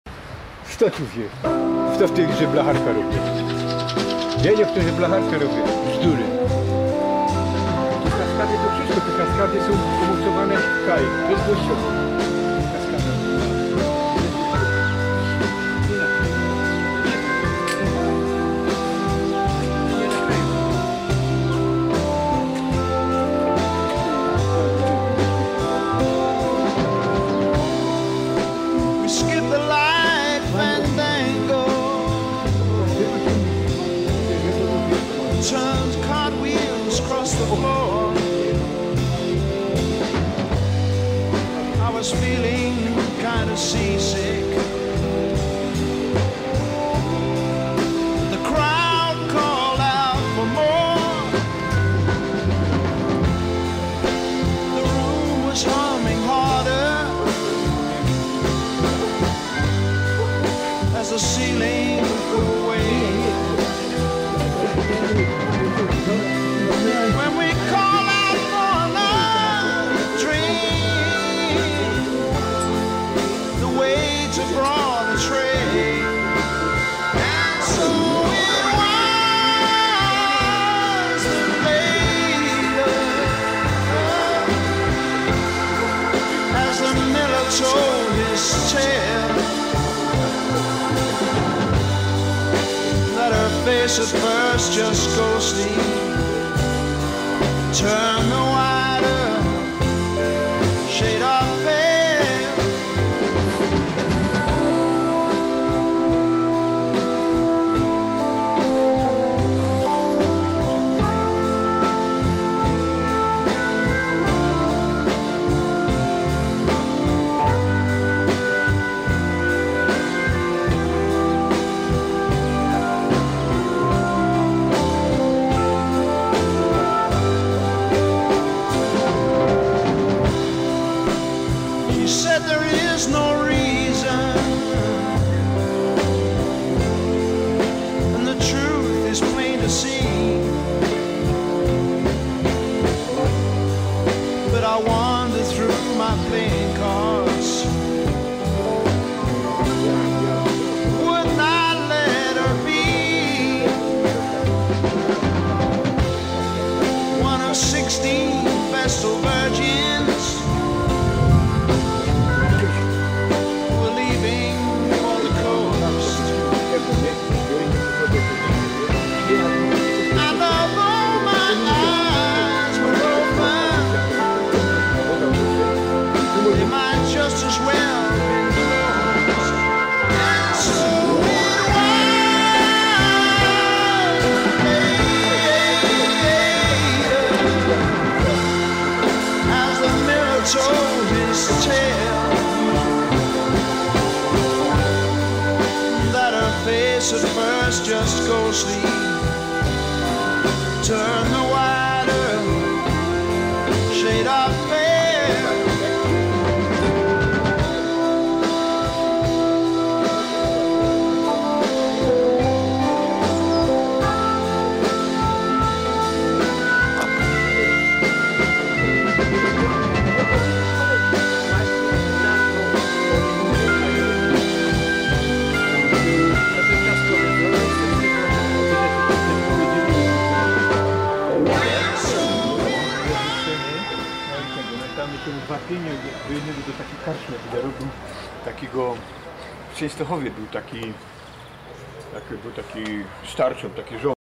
Hobby - Bildhauer und - Maler HUBERT PYTEL
Hubert Pytel wohnt in Tichau (Tychy ) bei Kattowitz.
Er ist ein Hobby - Bildhauer und - Maler.